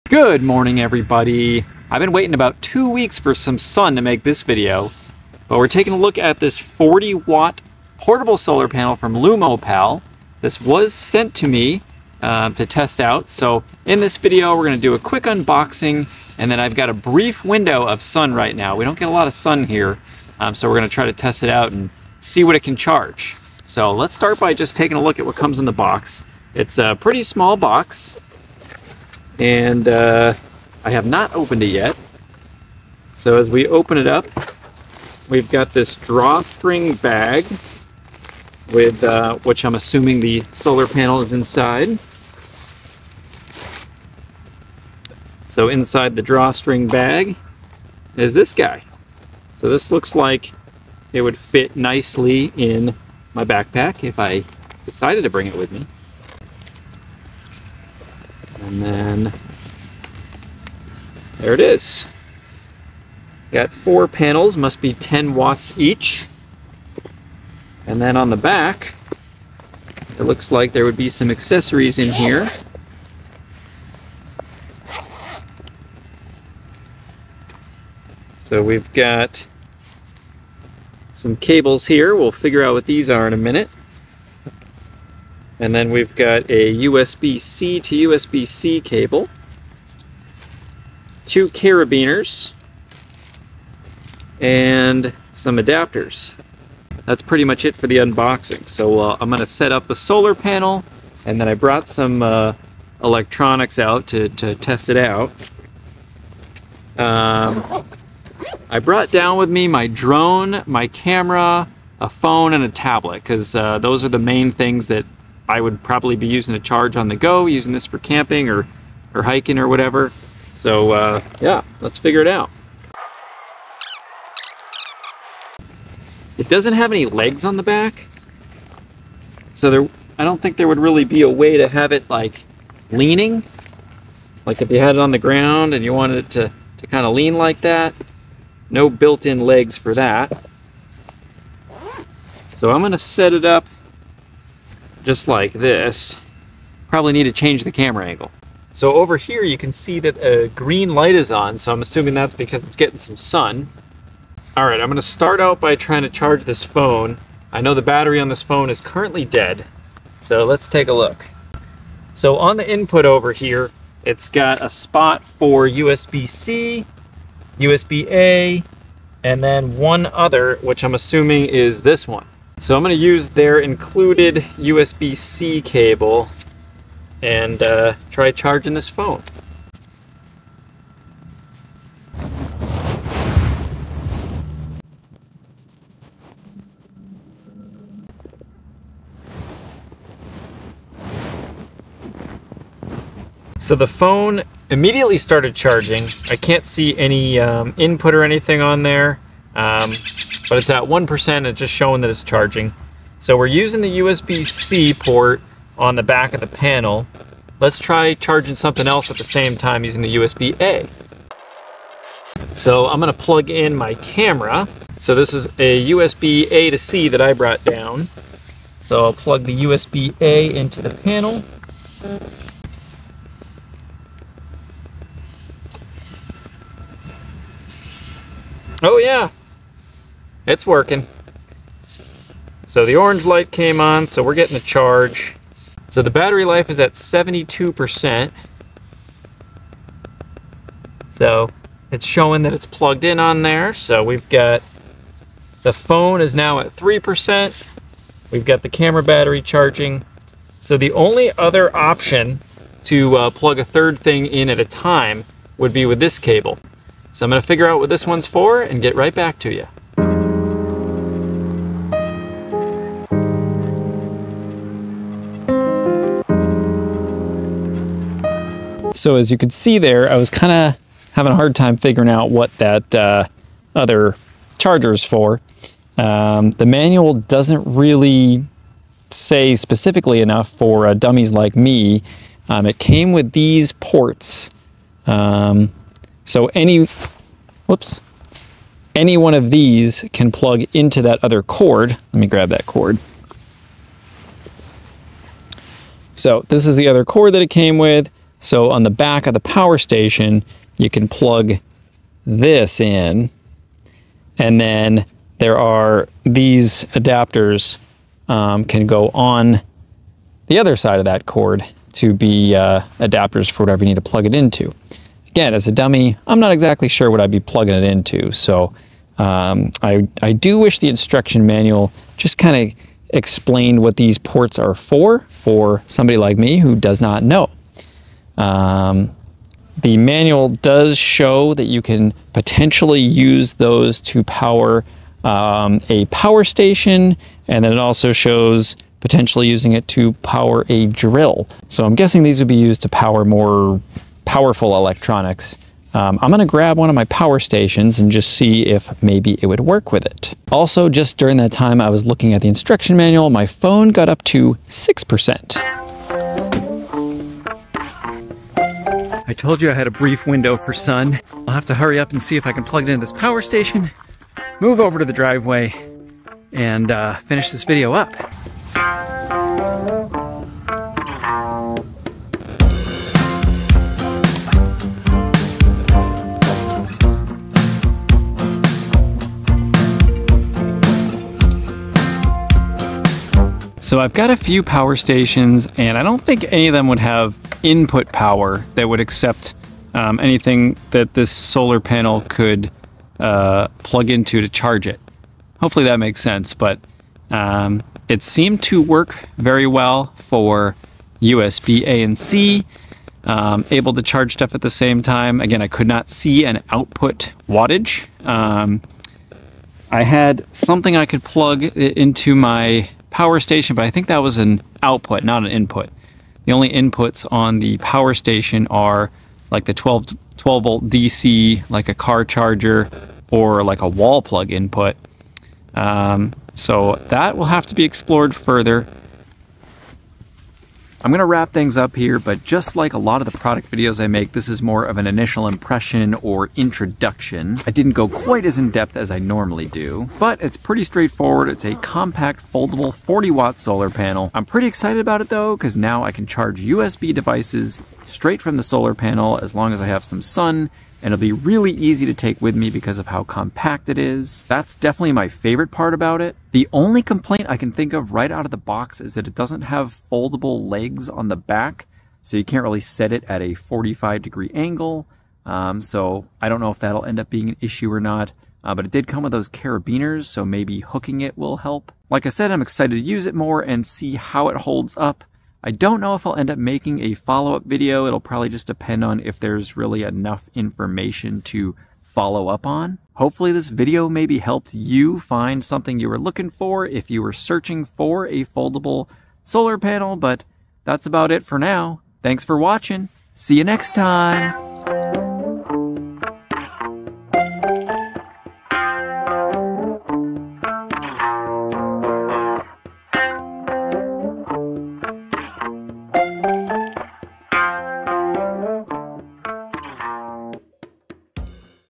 Big Power, Tiny Package: Lumopal 40W Foldable Solar Panel Review
Here's a quick introduction to this 40w solar panel from Lumopal. This item was sent to me for an honest review, however, this video is not sponsored, nor was I told anything to say or include in the video.